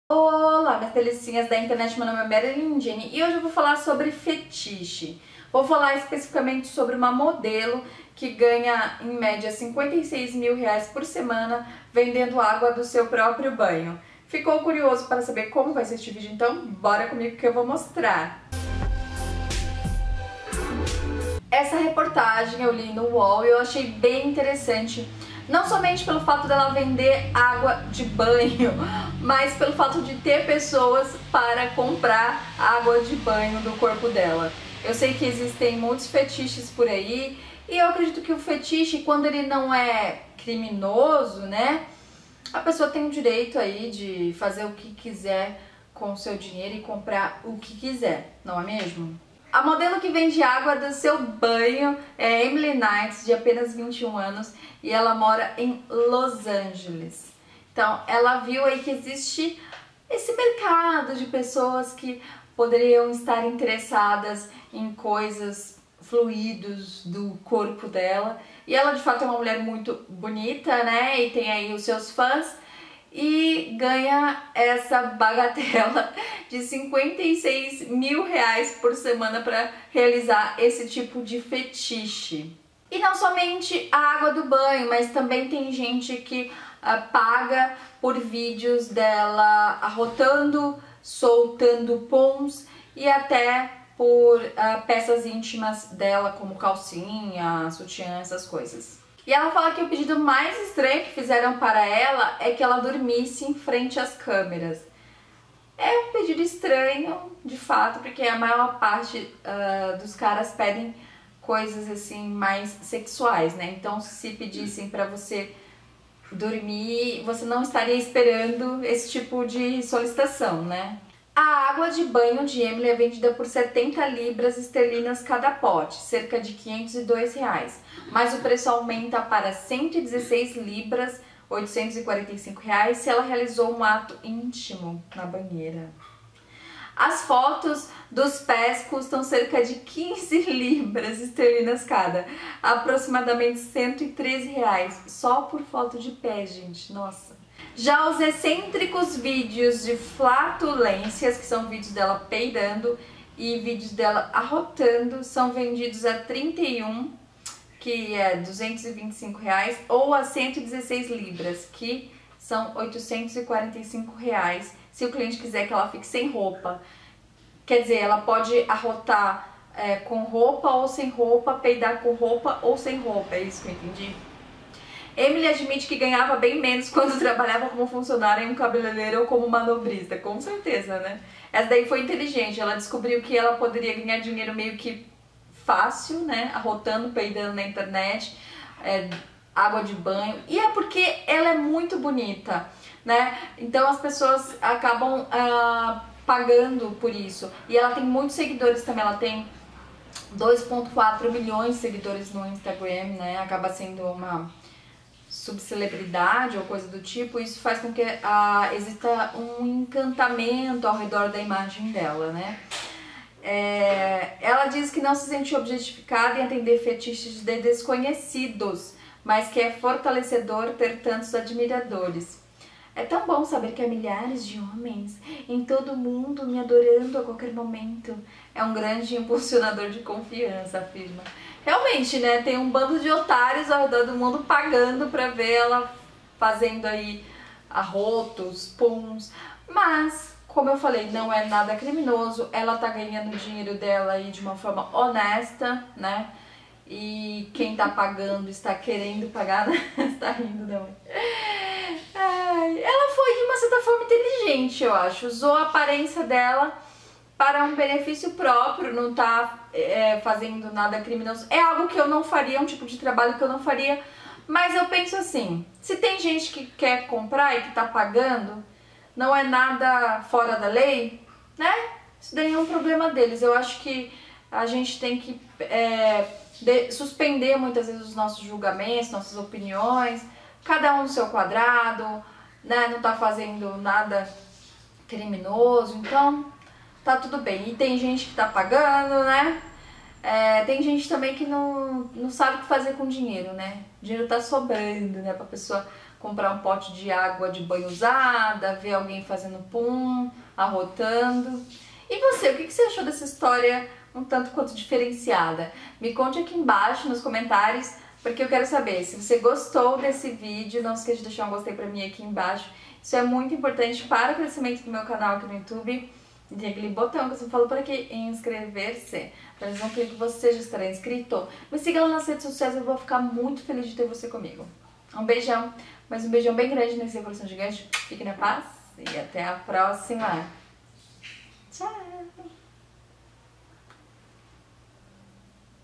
A MULHER QUE VENDE ÁGUA DE SEU BANHO
Você compraria água de banho de alguém? Neste vídeo vou falar de uma mulher que vende água de banho!

auto estima